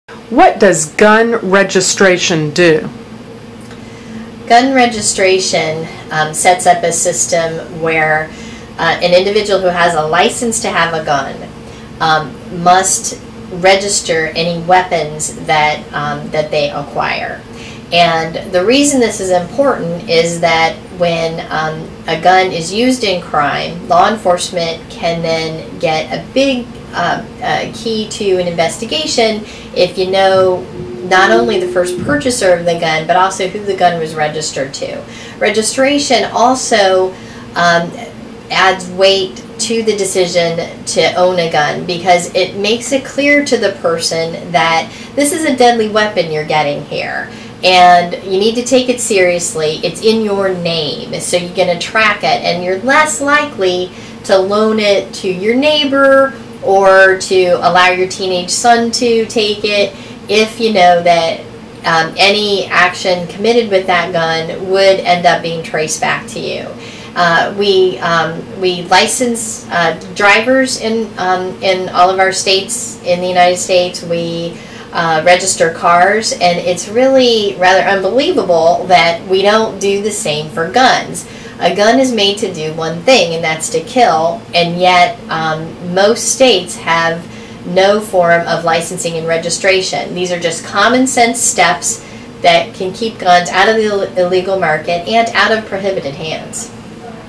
Gun Facts 101: Gun Registration
Jackie Kuhls answers common questions about guns and gun laws.

Q: What does gun registration do?